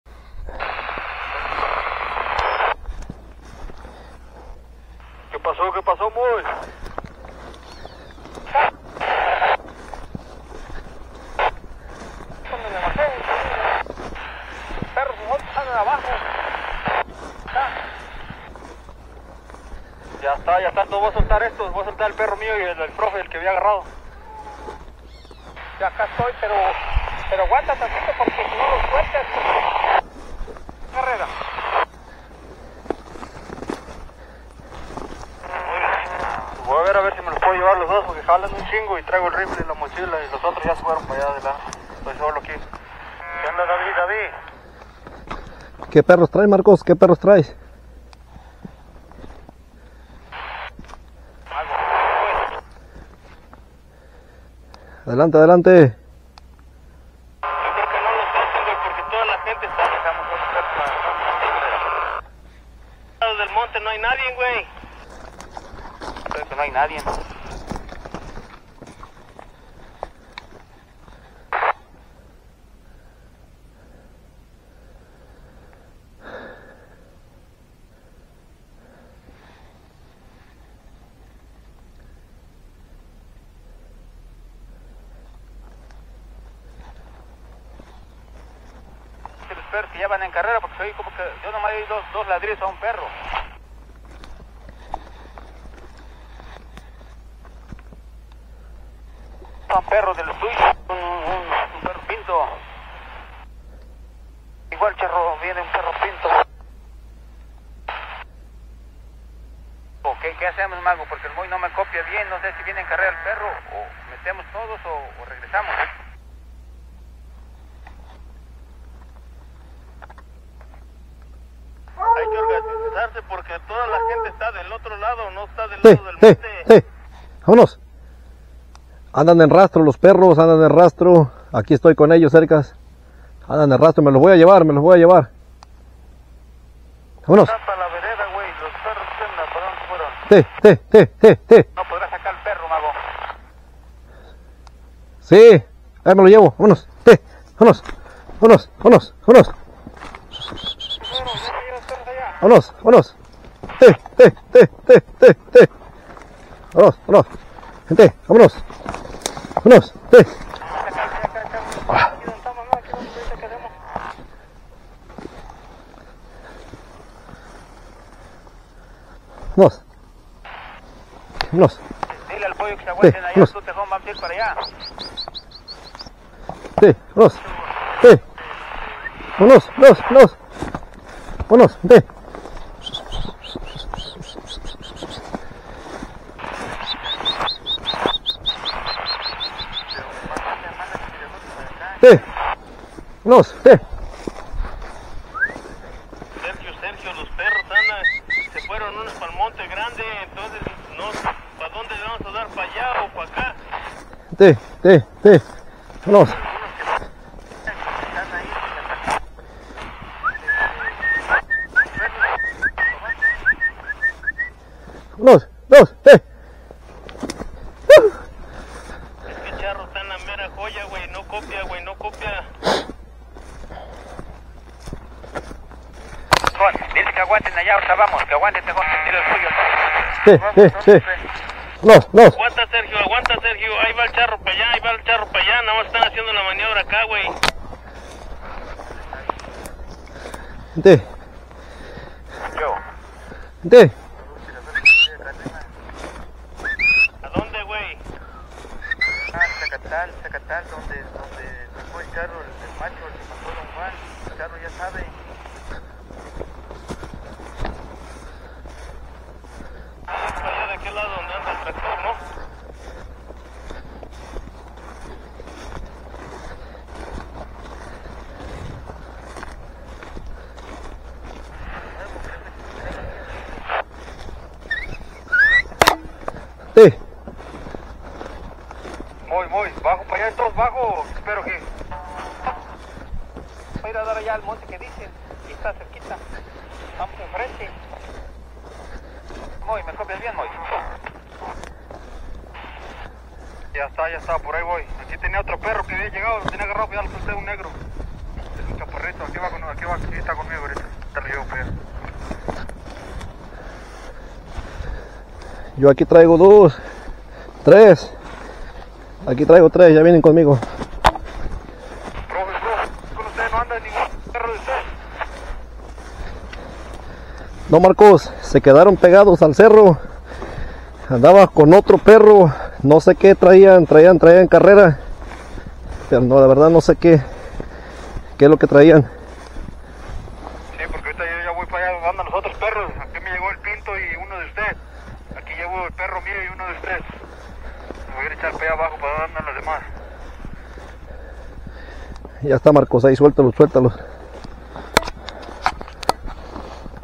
Se arma otra carrera de Venado amigos
Los perros trabajando amig@s en carrera de venado se recomienda el uso de audífonos para una mayor experiencia
VIVA LA CAZA LEGAL
EL GUAPO
EL TARZAN
LOS CAZADORES MC